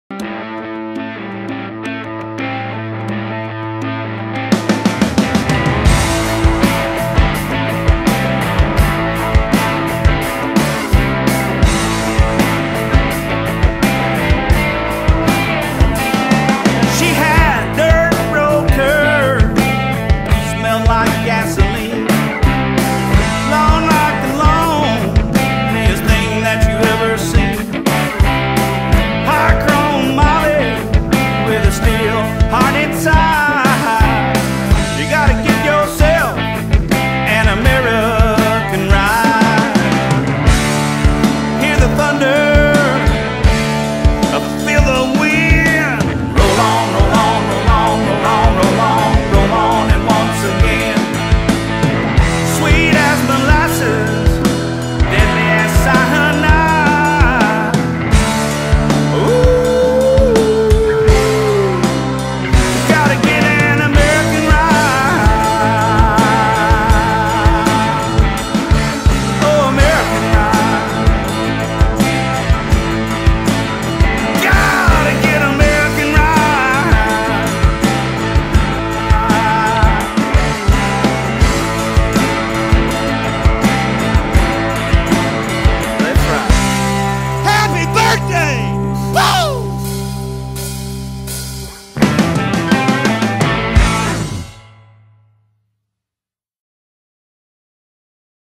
Smoky Mountain Harley-Davidson's 10th Birthday Bash
A great look back on our 10th year Birthday Bash celebration. Thanks to all for the support over the years, thanks to those who helped put on the celebration, and to everyone that made it out to the party. Until our paths meet again, Enjoy the Ride… Ride Safe!